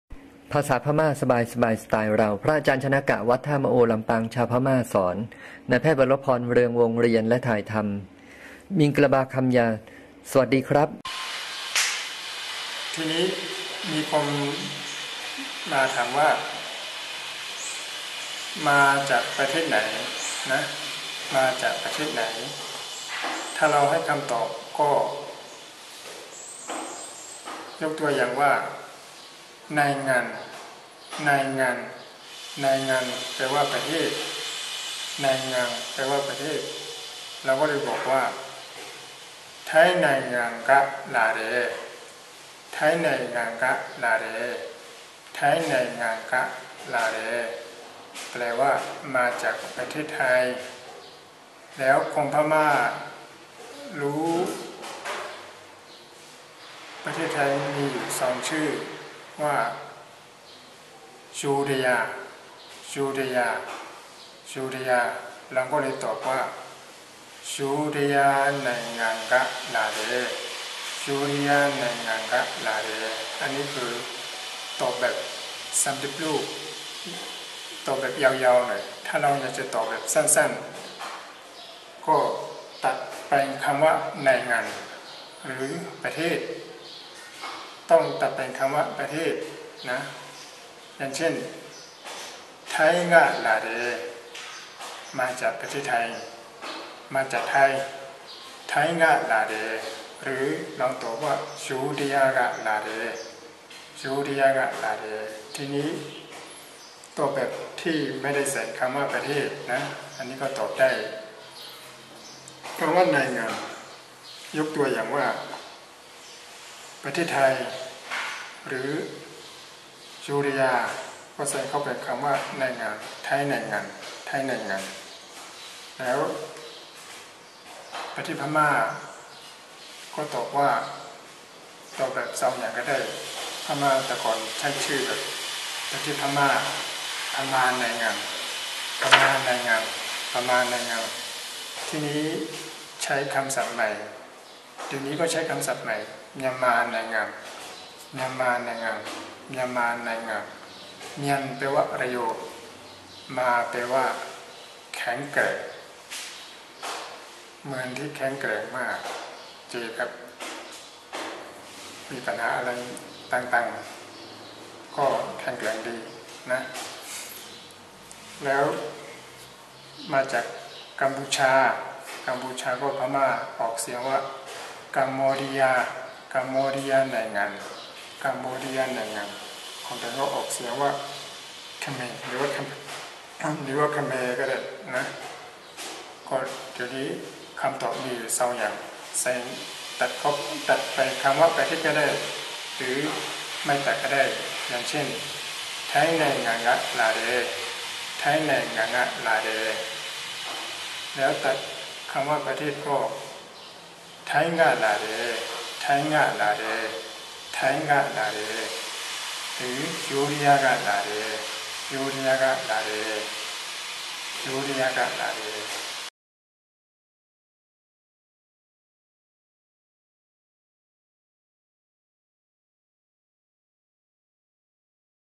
ภาษาพม่า ตอน20
ภาษาพม่า (เมียนมา) ตอน 20 - มาจากประเทศไหน // 'Learn Myanmar' for Thai speakers. CC: BY-NC-SA // ภาษาพม่าเรียง "ประธาน+กรรม+กริยา", คำขยายส่วนใหญ่ ใส่ไว้ข้างหน้า // ไวยากรณ์พม่าคล้ายภาษาญี่ปุ่น และมักจะมีอะไรที่ตรงข้ามกับภาษาไทยเป็นส่วนใหญ่